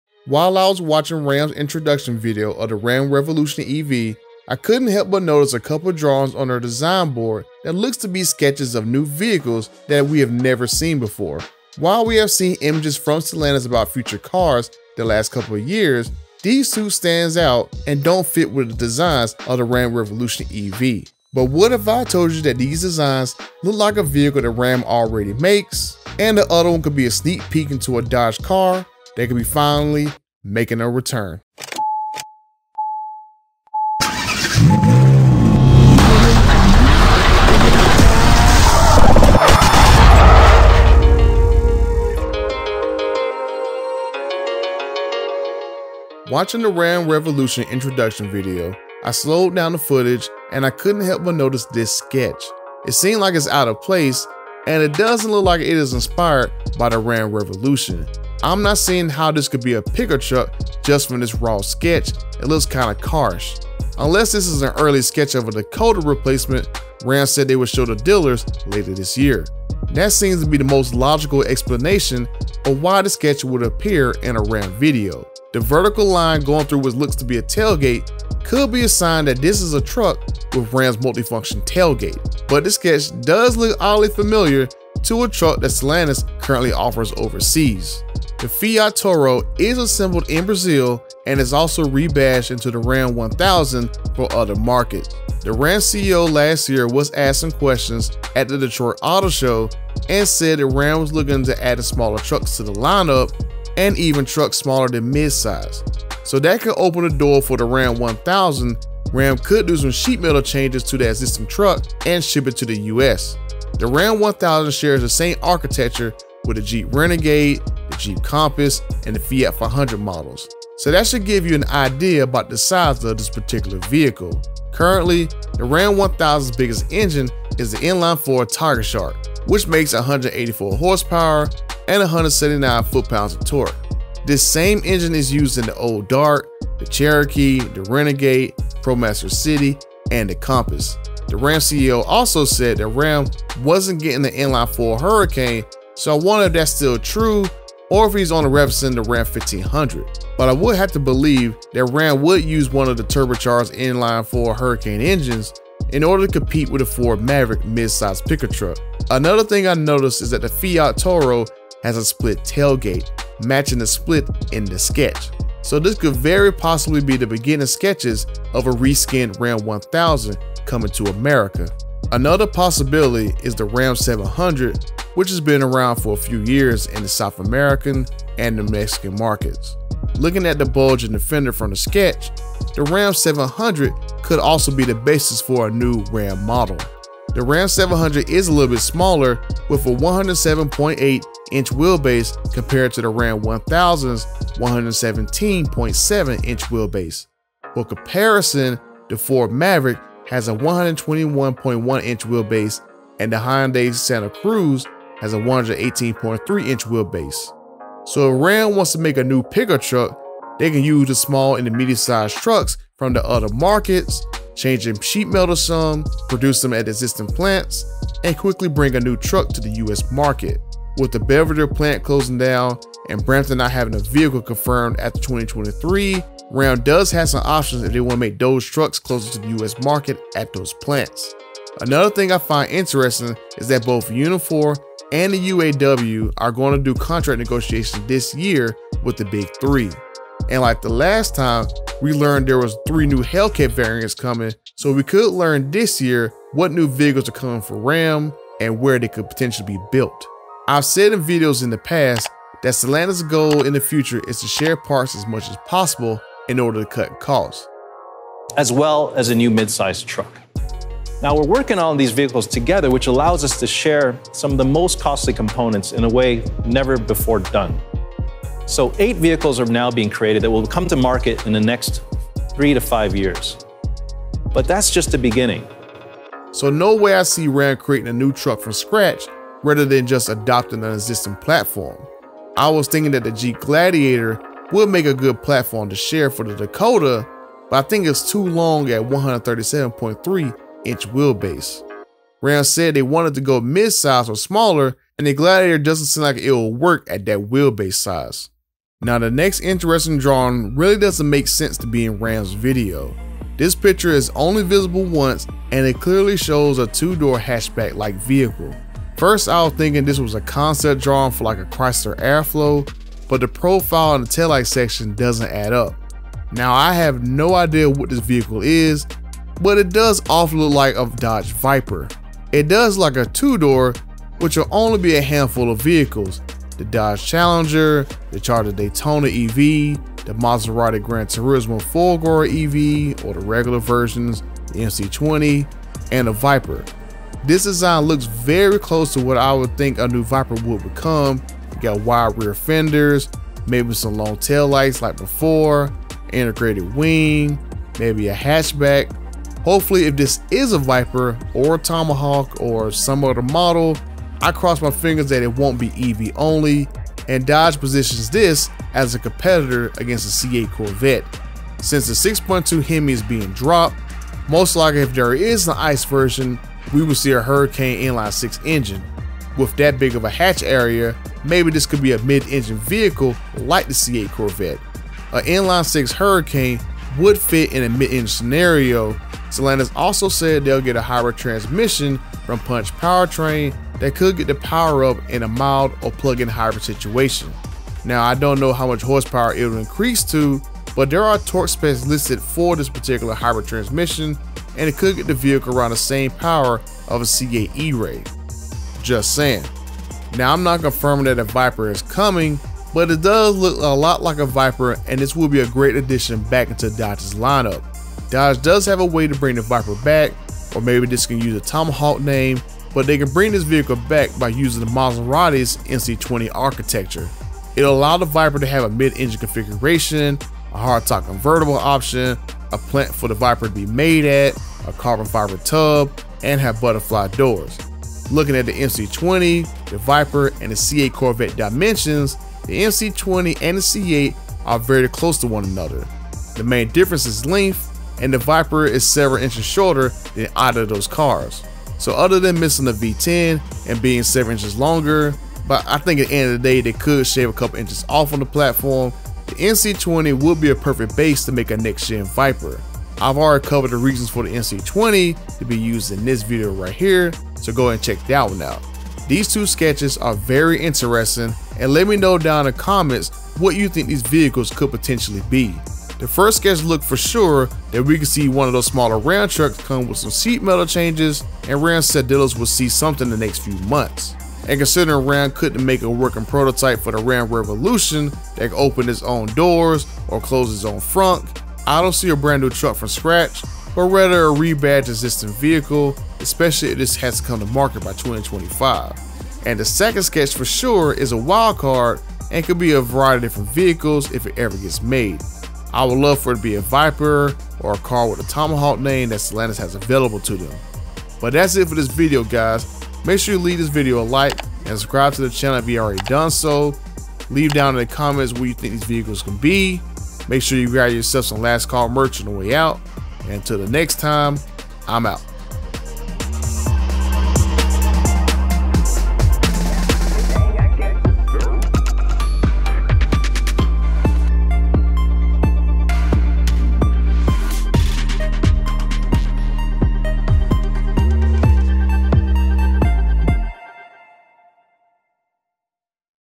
Did Ram leak the 2025 Dakota and Dodge Viper concept sketches?
The Ram Revolution concept made its debut at CES and it's unveiling may have accidently leaked concept drawings for new models coming.

Ram confirmed in 2022 that they were exploring adding multiple smaller pickup trucks to their lineup in the future and we could see an early design of it. Ram could do some slight styling changes to the Ram 1000 and 700 to bring it to the US market. But also in one of the quick shots from the Ram Revolution video, I spotted a sketch of a vehicle that looks to be a 2 door hatchback. Comparing it to previous Dodge Viper, this could be an early look at it.

0:00 New models leaked?
0:45 New Ram Dakota Sketch?
1:22 Ram 1000 redesign?
2:51 Ram 700 clues?
3:40 Unifor & UAW leaks
4:58 Viper Concept leaked?
6:00 EV, ICE, or hybrid?
6:40 MC20 Tomahawk
7:38 What concepts are these?